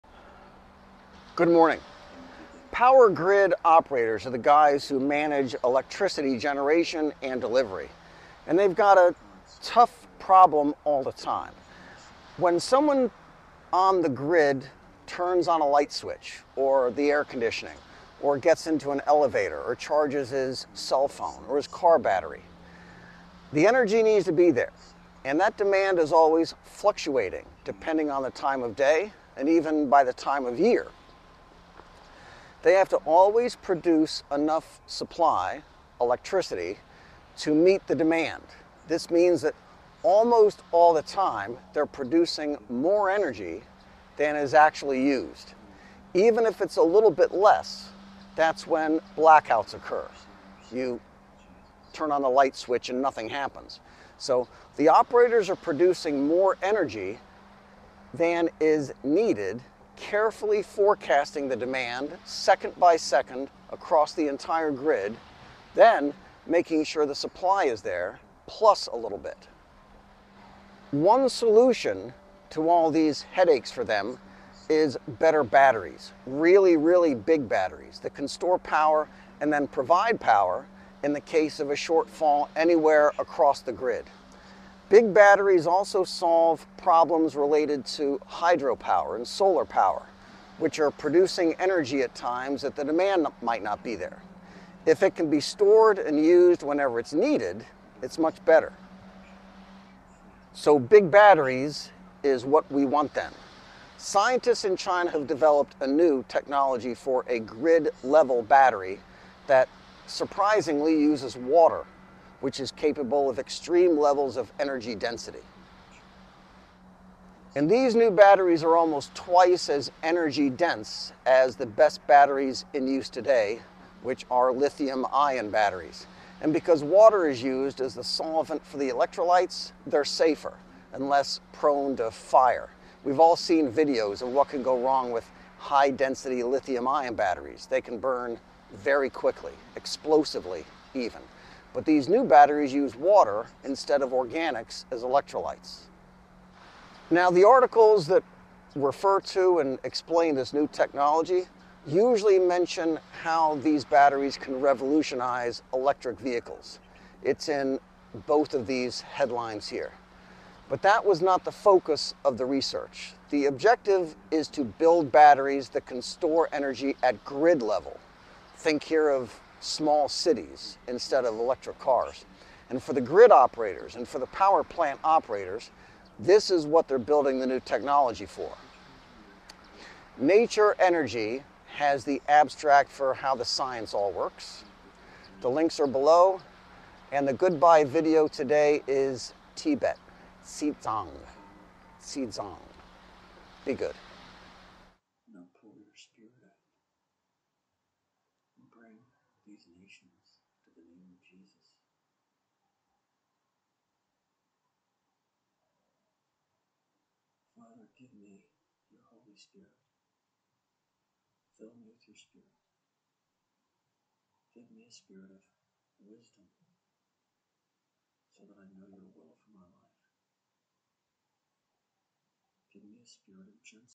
China builds new water battery to power entire electrical grids
Researchers have developed a revolutionary battery with more than twice the energy density of lithium-ion batteries.  At scale, these batteries can be employed across electrical grids, as backup systems for power plants.

Resources and links:

Chinese scientists create ‘water battery’ that can hold much more energy than lithium cells: study

China’s water battery has almost double energy capacity than lithium cells

Academic / Scientific abstract, Reversible multielectron transfer I−/IO3− cathode enabled by a hetero-halogen electrolyte for high-energy-density aqueous batteries